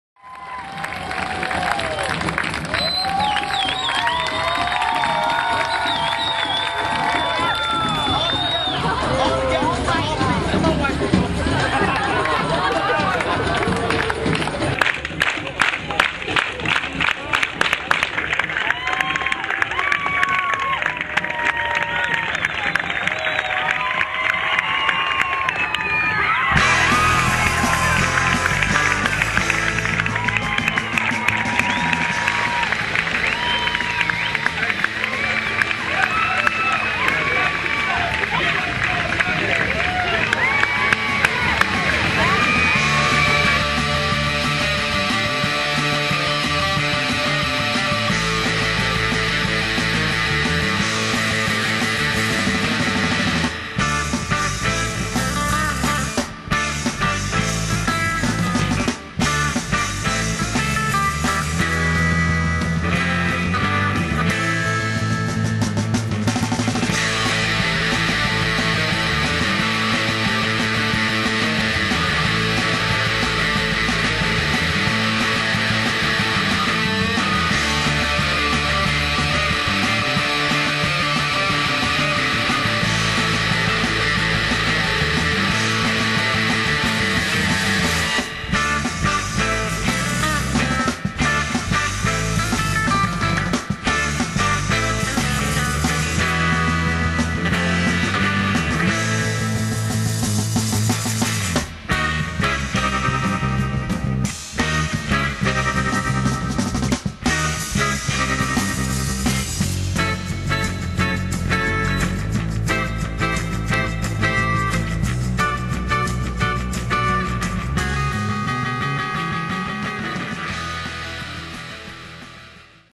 FULL FLIP over FIVE PEOPLE
BEACHSHOWTV captures this amazing GROUP OF ACROBATS at the HB Pier in Huntington Beach, California. Watch this talented performer do a FULL FLIP over five people.